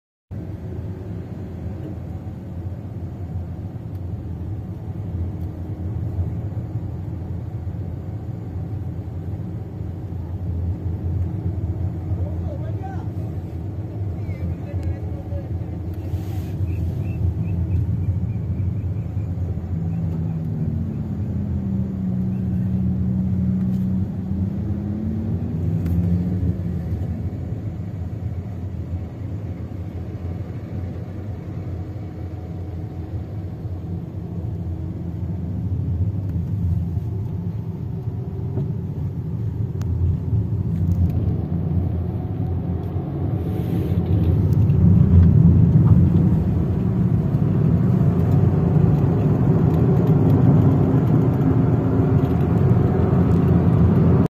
VIRAL MALAYSIA KEMALANGAN MELIBATKAN BEBERAPA BUAH KENDERAAN HARI INI 29/1/2022 JAM 10 PAGI
Kepada yang pulang ke kampung atau bercuti hari ni berhati hati dijalan raya, lokasi kejadian berlaku belum dikenal pasti